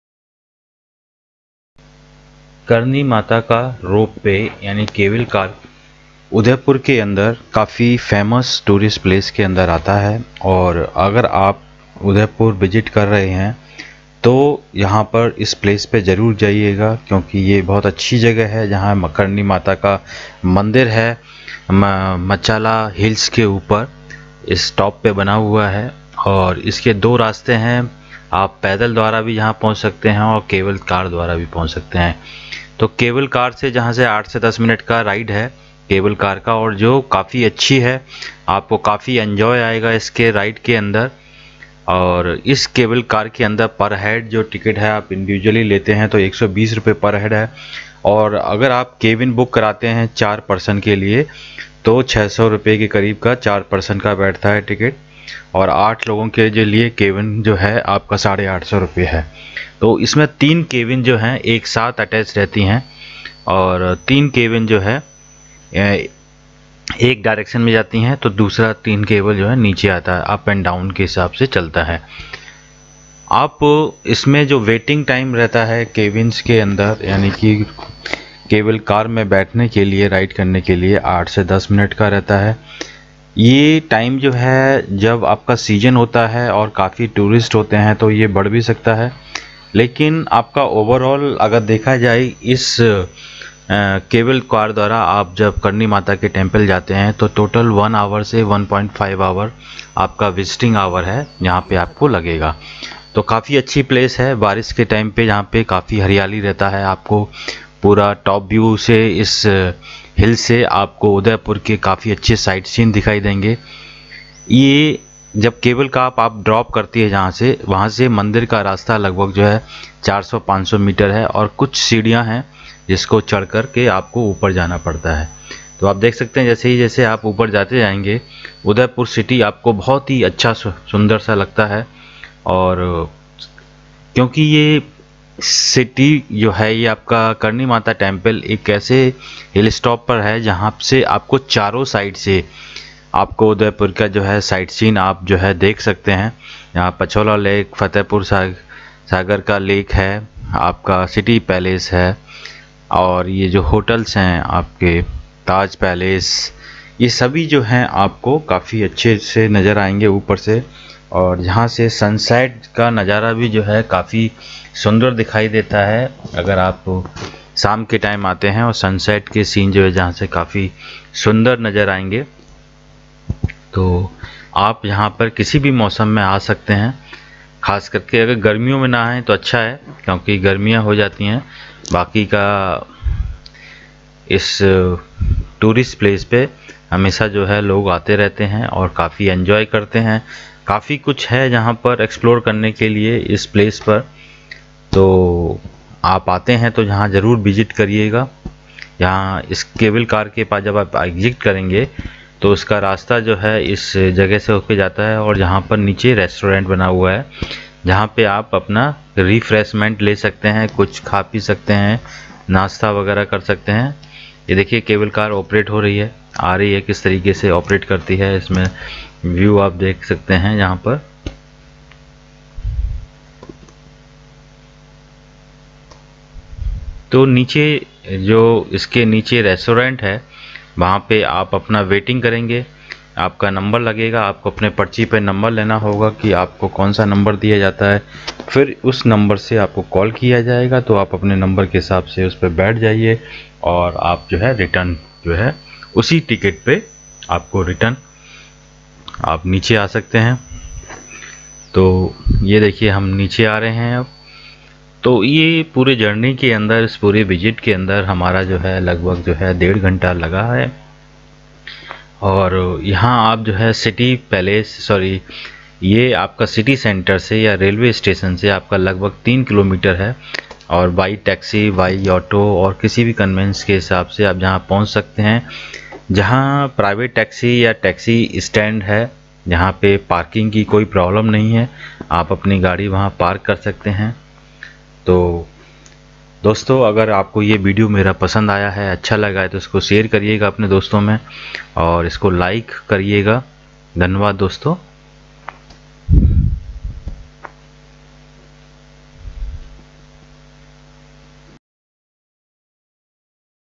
Karni Mata Temple Must to See in Udaipur City, Rajasthan | Karnimata Ropeway/Cable Car Tourist Place
Karni Mata Ropeway is a popular tourist attraction in Udaipur city which connects Karni Mata Temple at Machhala Hill Top by a cable car. You can reach at Karni Mata temple by walk or can reach by cable car.  There are three package for cable car, one is family cabin for 4 person in Rs. 600/- (approx..), second is family cabin for 8 persons in Rs. 850/- 9approx.) and third option is individual charge Rs. 120/- per head.The cable cars ferry sightseers on 5 to 7 minutes short trips up the hill. Three cable cars are attached together. Waiting time is 8 to 10 minutes depending on rush. From the cable car and from top of the temple can see the best views of Udaipur including Pichola Lake, Fateh Sagar Lake, Magnificent City Palace Complex, Sajjangarh Fort and landscapes of Aravali Hills on the other side of lake. There is also a view point which is the popular spot for seeing the sunset over the city.

Advisable to carry water bottle , use light comfortable dress , wear good walking shoes while going to top . Carrying babies is not a problem here. Rainy and winter time is good to visit this place.
Total visit time 1 to 1.5 hrs. required depends upon rush. This place is almost 3 km from the city railway station and can reach here by private car, taxi, auto or bus. Car parking is no issue here.
This video is prepared and presented by guideuncle.